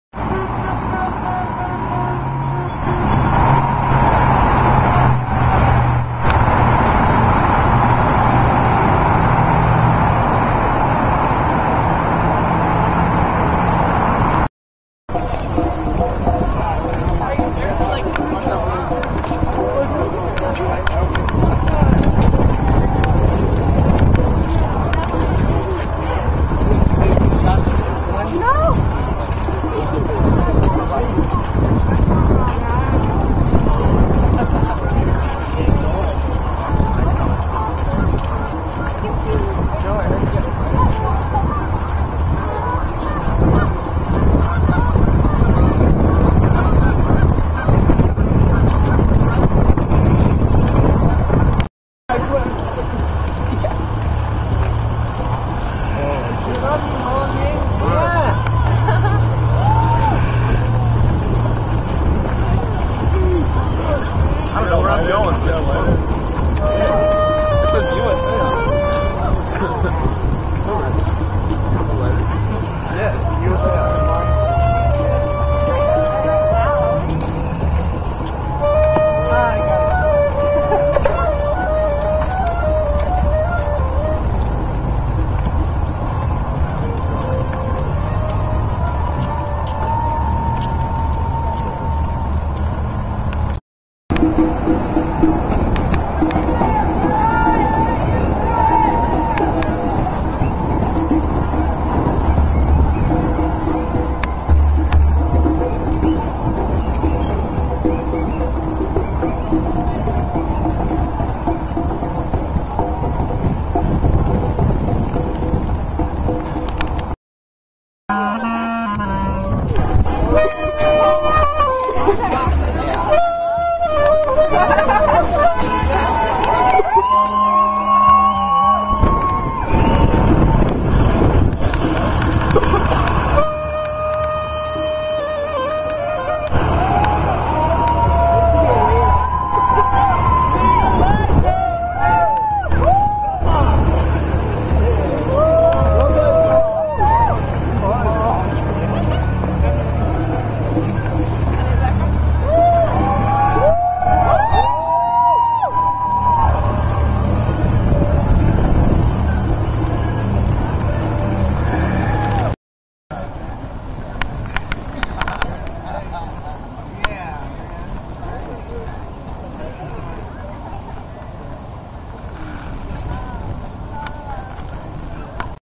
Marijuana March Mpls - May 2008 (remastered)
A collage of the Minneapolis 2008 Global Marijuana March, in reverse. I upgraded this video to 720p HD using Youtube's video editor. This video begins near the end of the march and ends at the start, yes its in reverse.